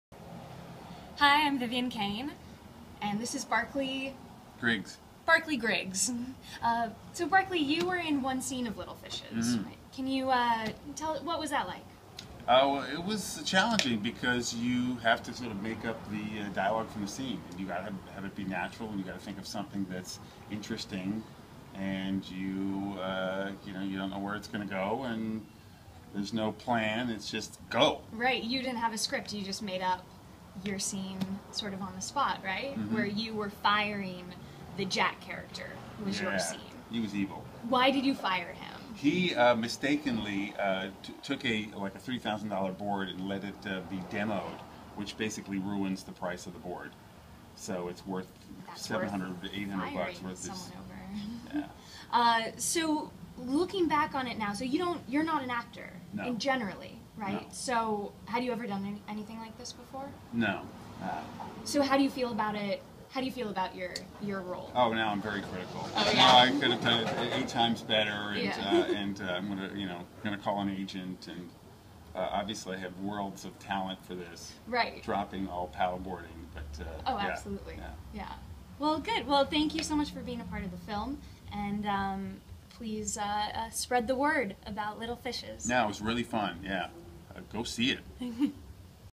Vivian Kane and Barklie Griggs Talk Little Fishes
Interview for new film, Little Fishes.